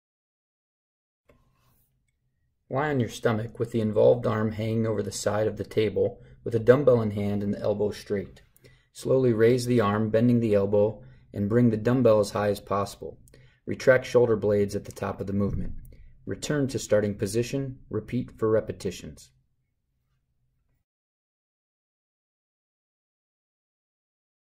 Prone Shoulder Row - Shoulder Strengthening & Scapular Conditioning
DrLinvilleDPT.com - Sports Physical Therapist

Lie on the stomach, with the involved arm hanging over the side of the table, with a dumbbell in hand and the elbow straight. Slowly raise the arm, bending the elbow and bring the dumbbell as high as possible. Retract shoulder blades at the top of the movement. Return to starting position. Repeat for repetitions.